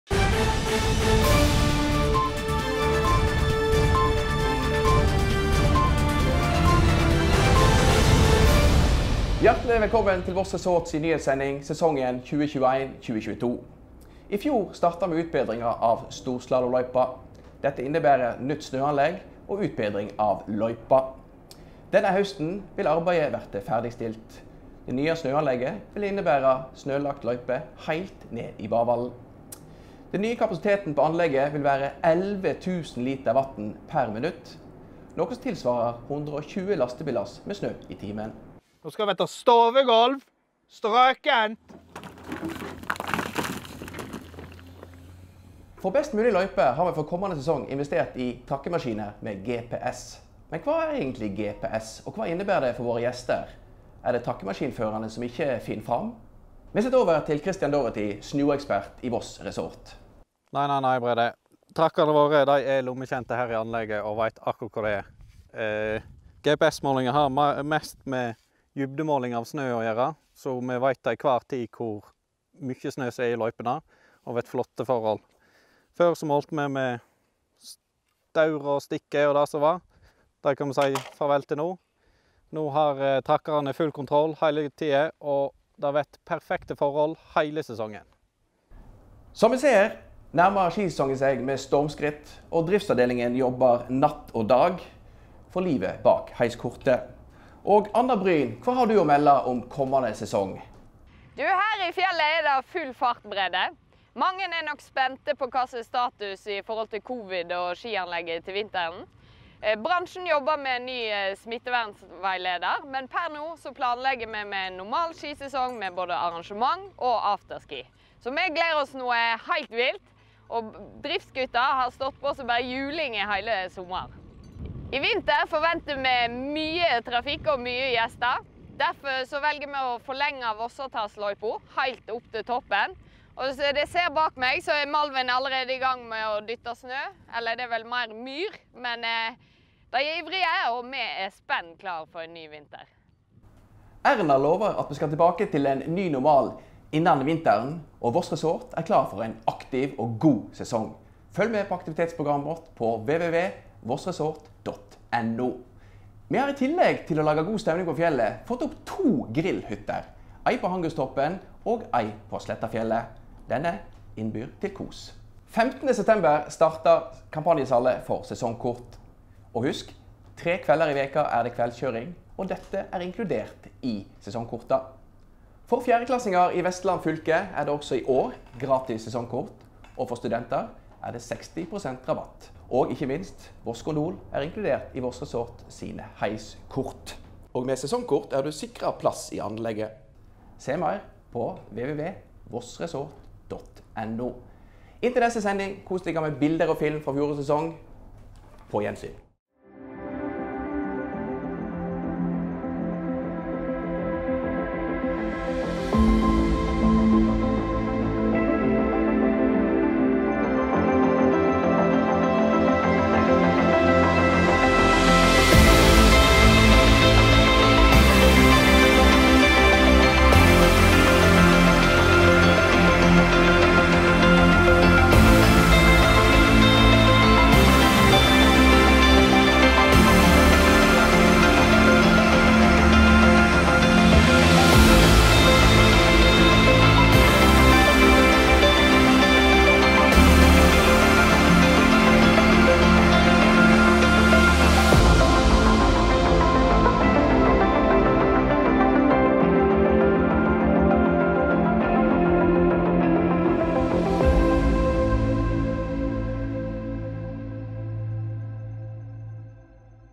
Voss Resort nyhetssending, september 2021
Me bryt inn med ei ekstra nyhetssending i høve start av kampanjesal på sesongkort for sesongen 2021/2022. Sjå meir her